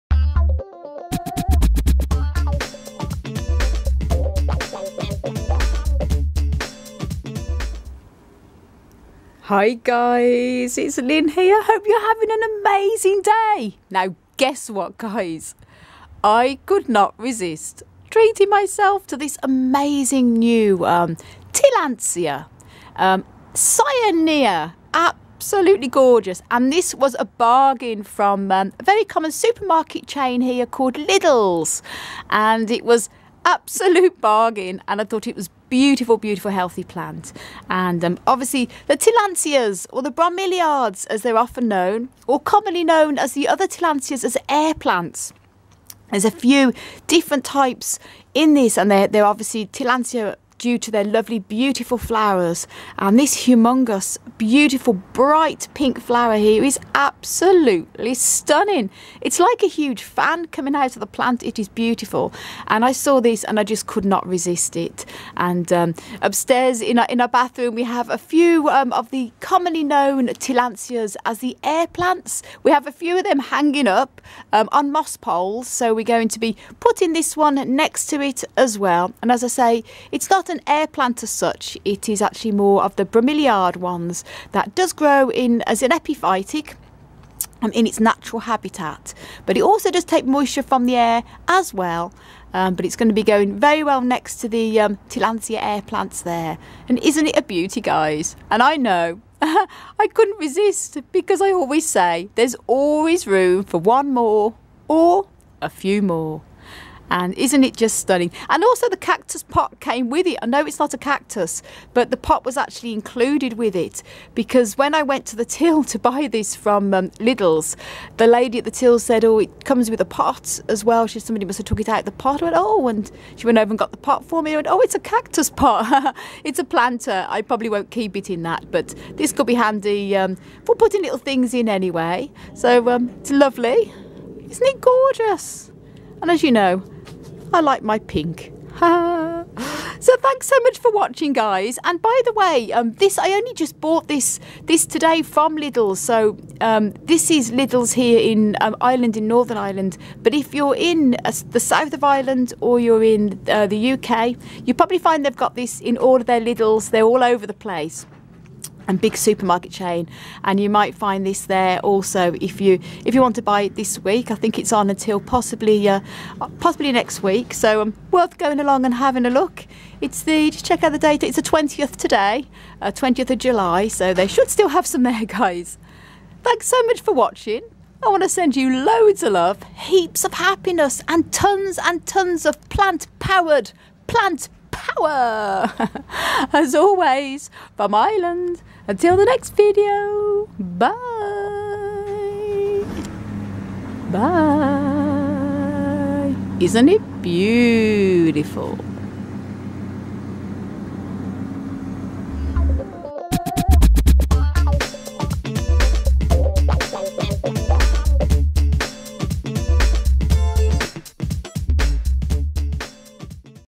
My Tillandsia cyanea 'Pink Quill' - Air Plant Bromeliad BARGAIN from LIDL
Hi Guys :-D I just could not resist this beautiful Tillandsia cyanea 'Pink Quill' -Bromeliad Plant BARGAIN from my local supermarket LIDL :-D